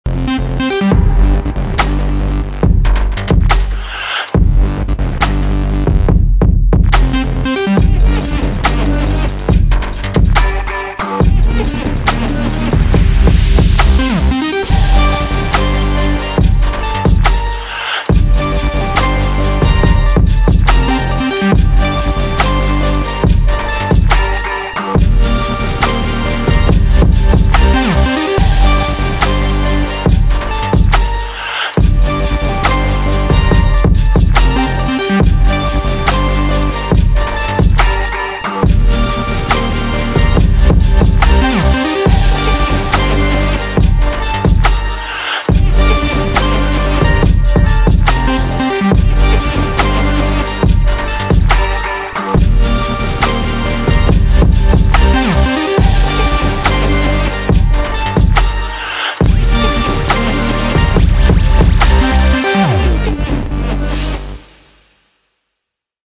3D Printing in Car Design – the 2020 MINI John Cooper Works GP
How Mini used 3Dprinting in the design of the new JohnCooperWorksGP. Full story